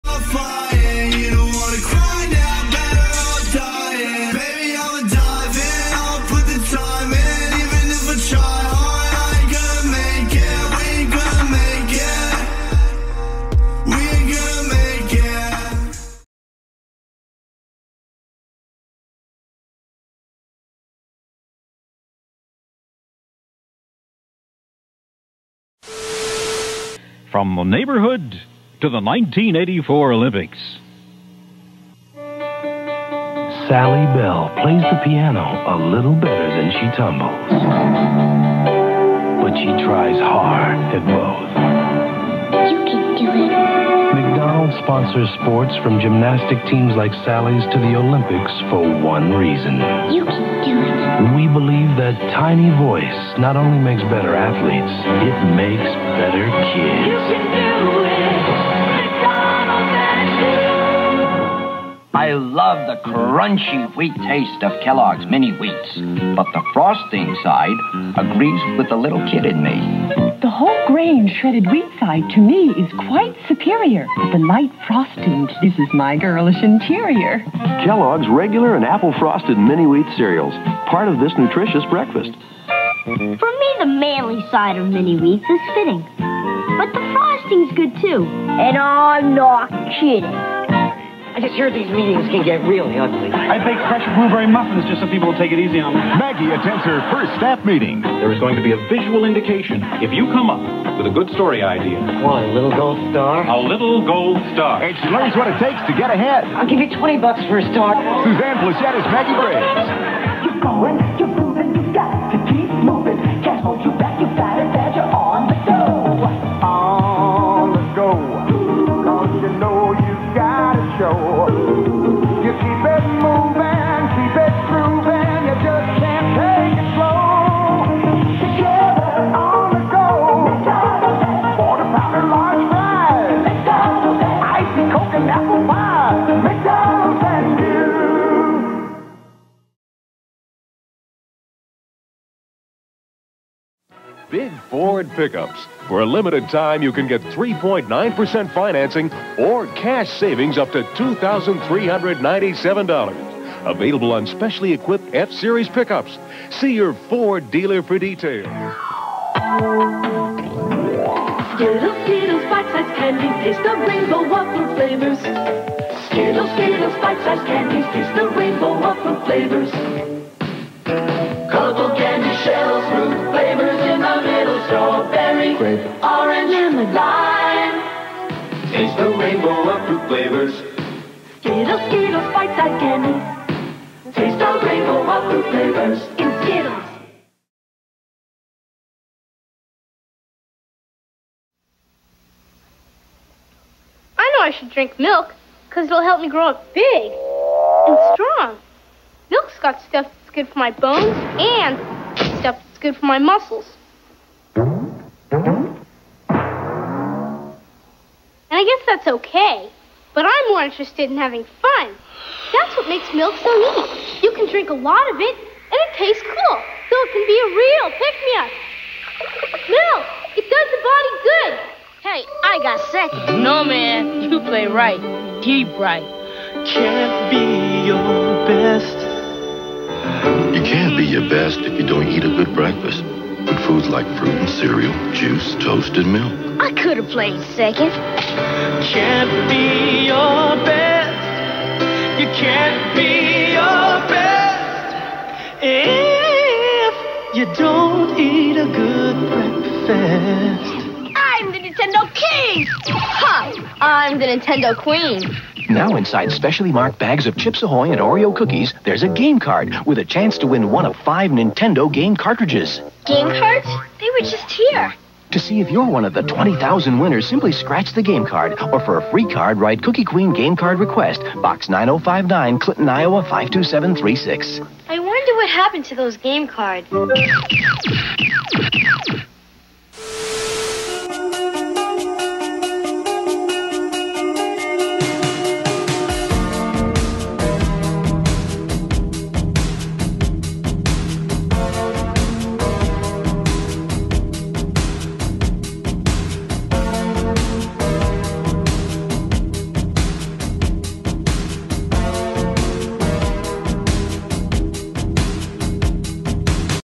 Pauly’D The Cinema Dude retro 80s fan video #80
In this video I added commercials from 1984, 1987, 1989